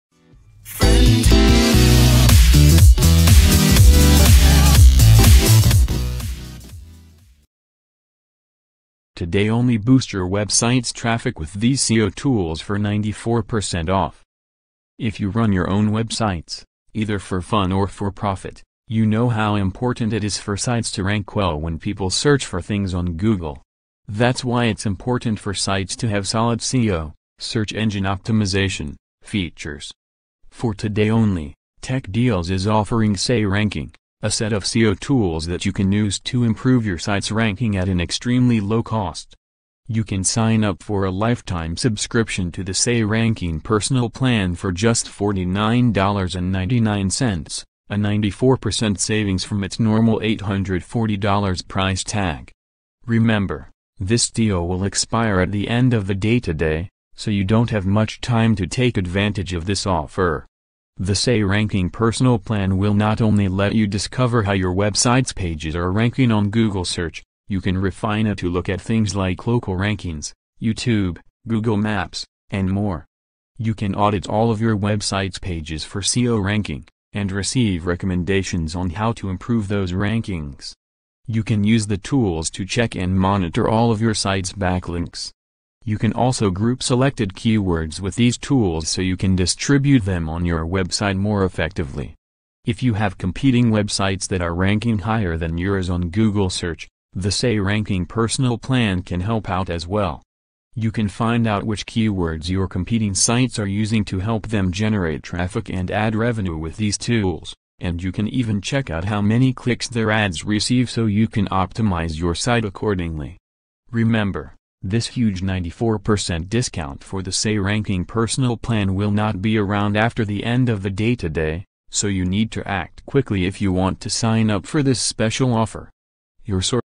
Today Only Boost your web site’s traffic with these SEO tools for 94 percent off
If you run your own web sites, either for fun or for profit, you know how important it is for sites to rank well when people search for things on Google. That’s why it’s important for sites to have solid SEO (search engine optimization) features. For today only, Tech Deals is offering SE Ranking, a set of SEO tools that you can use to improve your site’s ranking at an extremely low cost.

You can sign up for a lifetime subscription to the SE Ranking Personal Plan for just $49.99, a 94 percent savings from its normal $840 price tag. Remember, this deal will expire at the end of the day today, so you don’t have much time to take advantage of this offer.

The SE Ranking Personal Plan will not only let you discover how your web site’s pages are ranking on Google search, you can refine it to look at things like local rankings, YouTube, Google Maps, and more. You can audit all of your web site’s pages for SEO ranking, and receive recommendations on how to improve those rankings.  You can use the tools to check and monitor all of your site’s backlinks. You can also group selected keywords with these tools so you can distribute them on your web site more effectively.